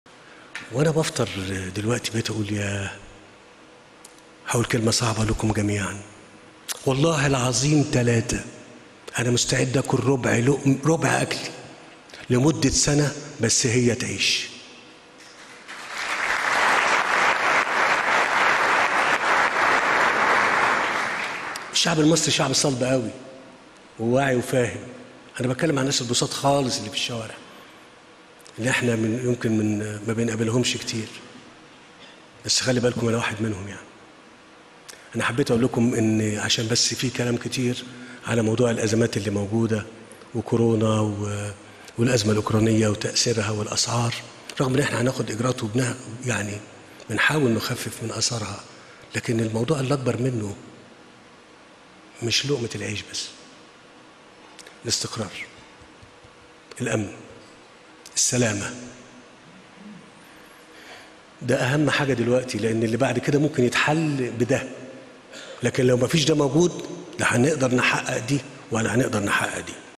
الرئيس السيسي: مستعد أكل ربع أكلي لمدة سنة بس مصر تعيش
قال الرئيس عبدالفتاح السيسي خلال كلمته في حفل إفطار الأسرة المصرية مستعد أكل ربع أكل لمدة سنة بس مصر تعيش
شهد حفل إفطار الأسرة المصرية، الذى يقام اليوم الثلاثاء  بمشاركة الرئيس عبد الفتاح السيسى رئيس الجمهورية،حضور لفيف من  المدعوين إلى مقر الحفل. وفيه هنأ الرئيس عبد الفتاح السيسي، خلال مشاركته في إفطار الأسرة المصرية، الشعب المصري؛ مسلميه ومسيحييه بالأعياد المباركة، مؤكدًا أن عهده مع المصريين دائما هو الصدق والإخلاص في العمل والنوايا، والتجرد من الانتماء إلا للوطن، ابتغاءً لوجه الله سبحانه وتعالى. وأعلن الرئيس عبد الفتاح السيسي ، رئيس الجمهورية ، عددًا من القرارات الاقتصادية التي تهدف إلى الاستمرار في المنظومة الصحيحة التي تنتهجها الدولة المصرية للنهوض اقتصاديا،